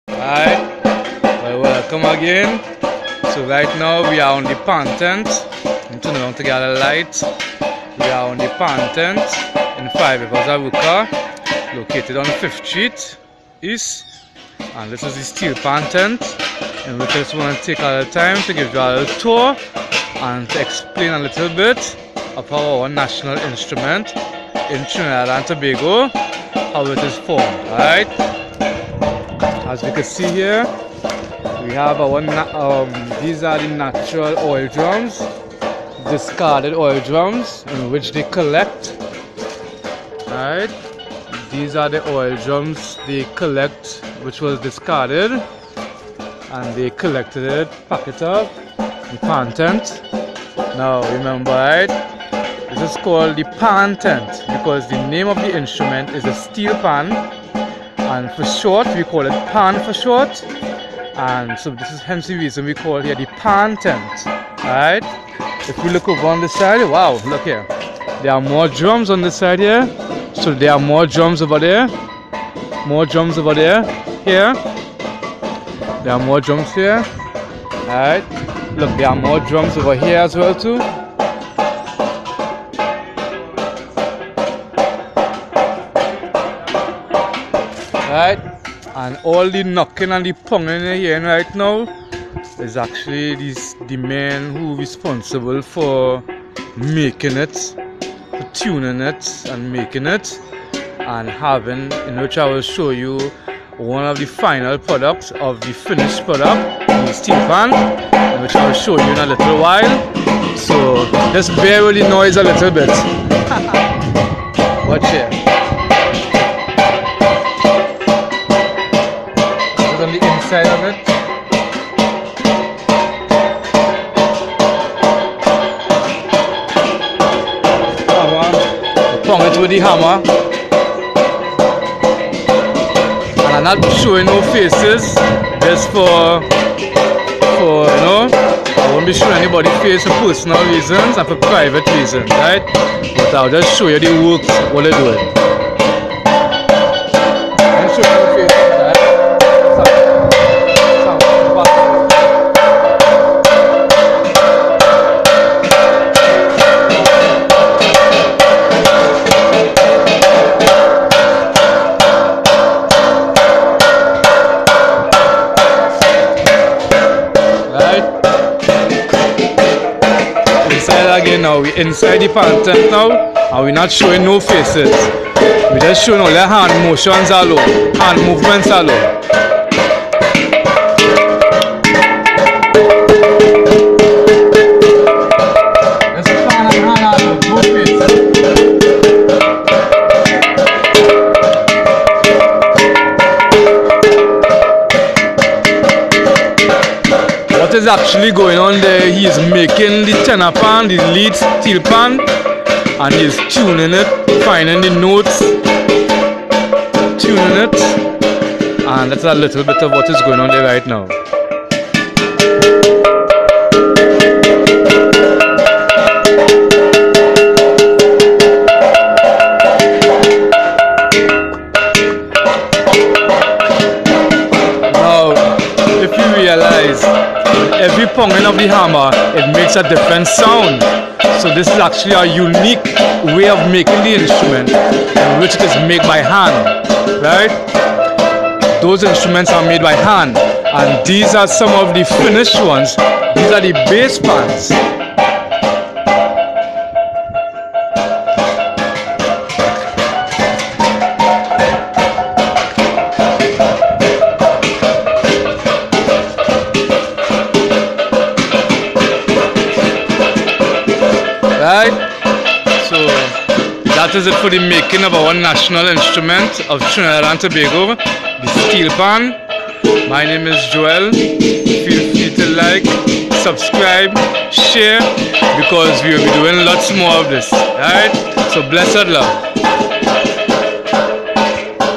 Making the steel pan - The national instrument of Trinidad and Tobago.
This is a very brief tour of our steel pan tent.It is where they make the steel drum.The national instrument of Trinidad and Tobago.